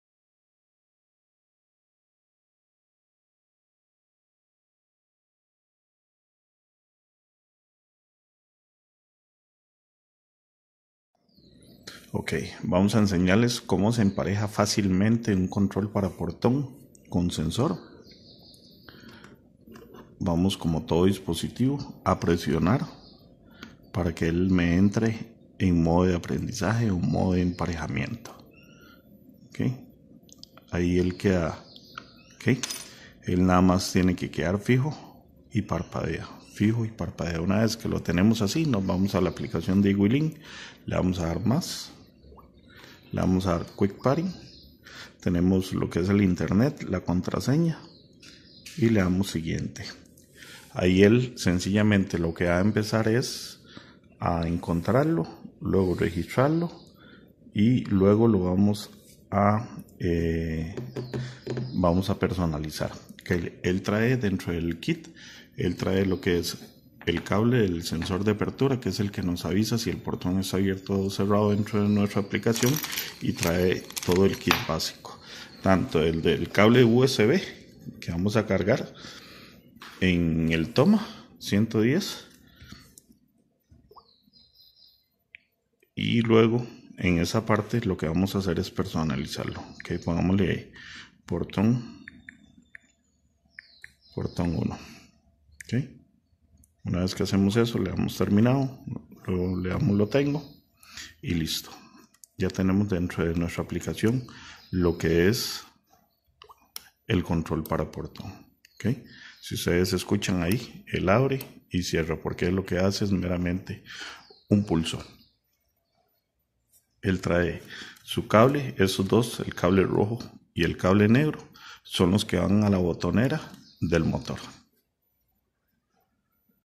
Configuración: Control para portón con sensor Duotech
Procedimiento para agregar a eWeLink nuestro control para portones eléctricos con sensor.